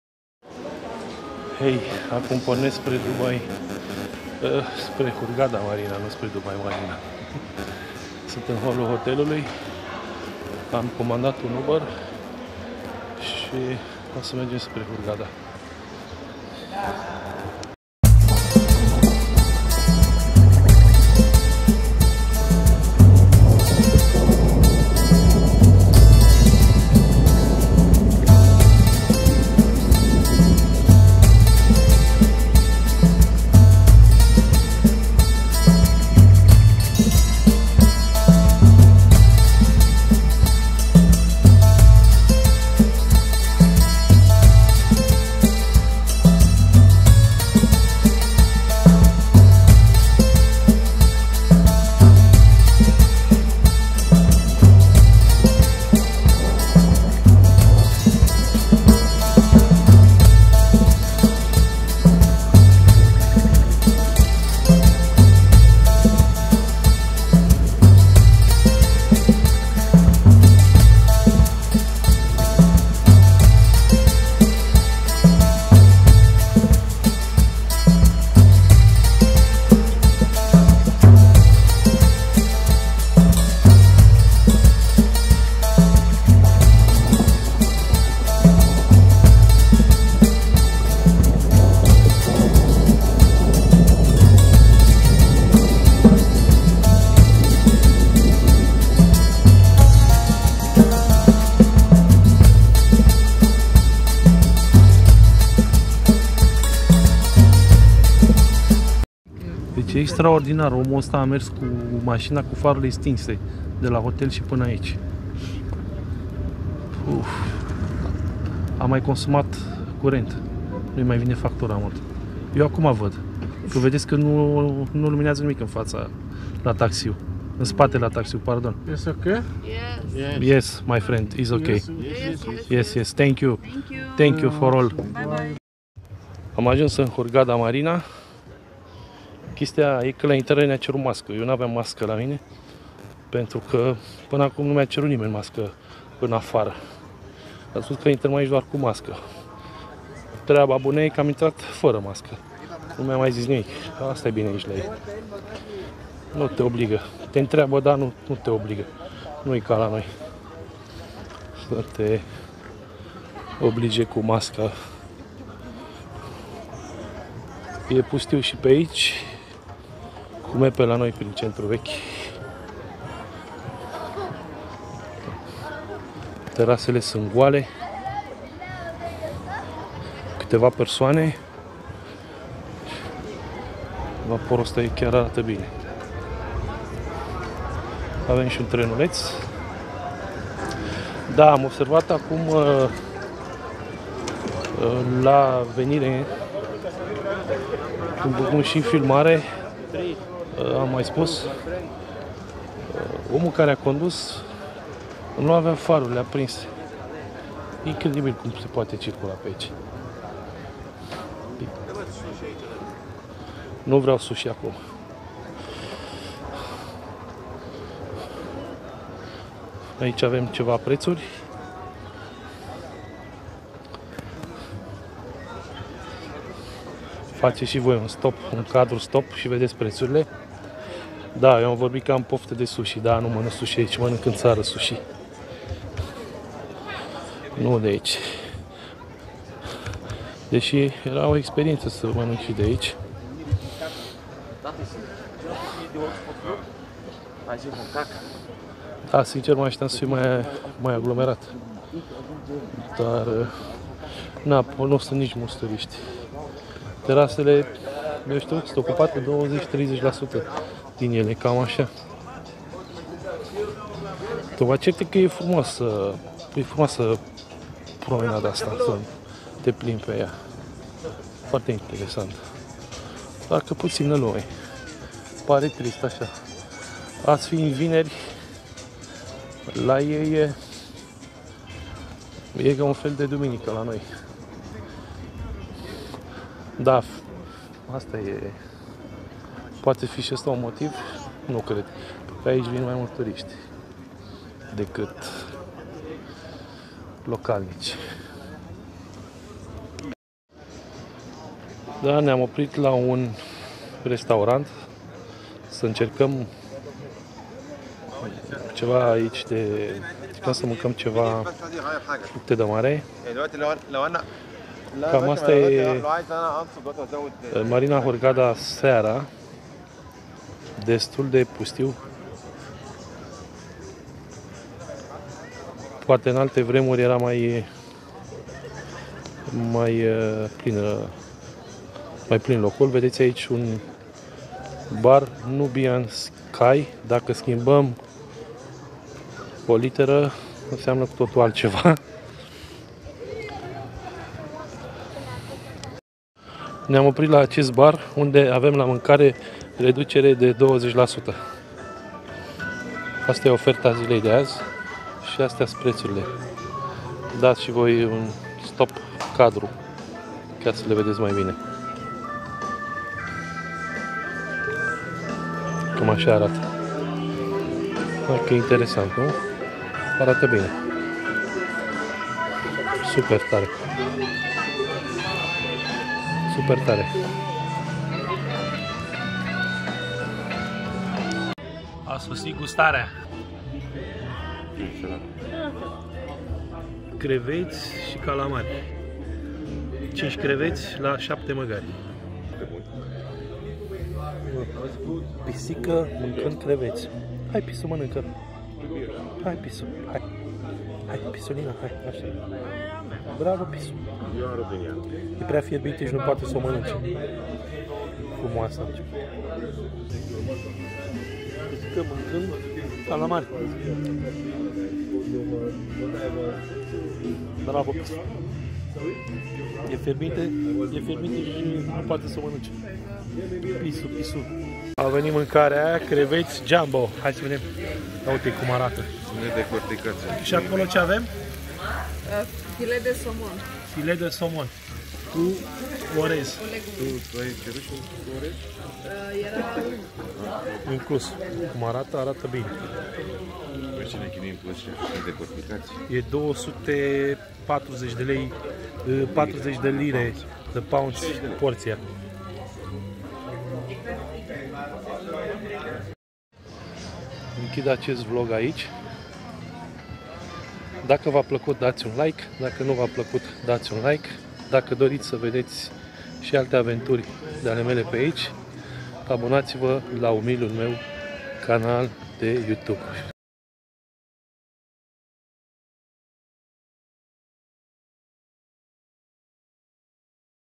Hurghada Marina/Plimbare la ceas de seara în plina pandemie!La intrare ti se cere sa porți masca!!!
Salutare!

În vlog ul de azi vom face o plimbare de seara prin Hurghada Marina și vom lua cina la un restaurant local!

M-ar bucura ff mult daca v-ati abona la canalul meu pt a ma însoți în toate călătoriile mele și pt a fi la curent cu toate noutățile!